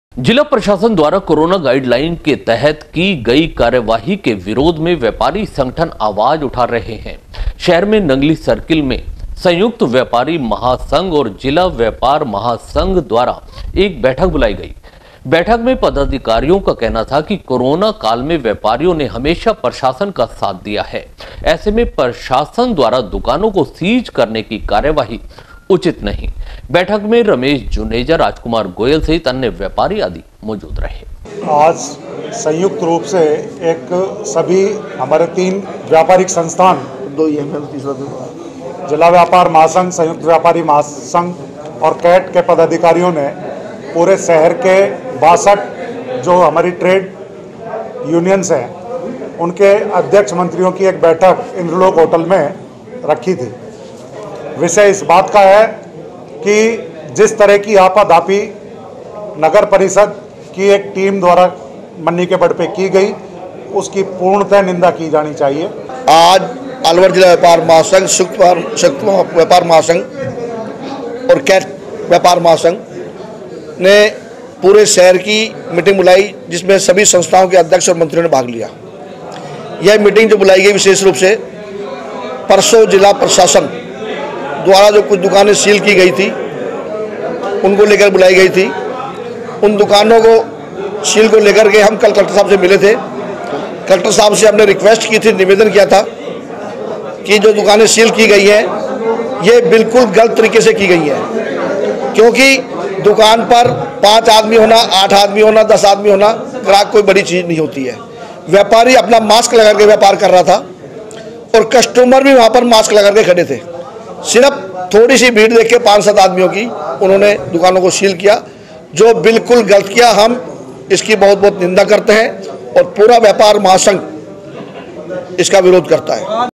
शहर के व्यापारियों ने दुकानें सीज करने का किया विरोध...
संयुक्त व्यापार और जिला व्यापार महासंघों ने की बैठक

प्रशासन की सख्ती पर जताया आक्रोश

व्यपारियो से नरमी बरतने की उठायी मांग